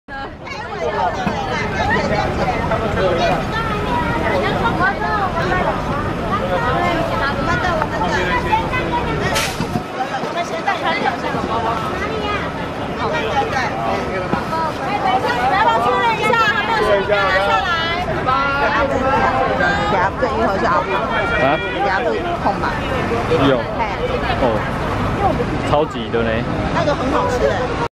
171210CC 台南永康●龍族遊覽車到達終點站18:23
感謝老天爺在落山風季撥出兩天 ~ 藍天碧海爆好天氣給我們玩得很開心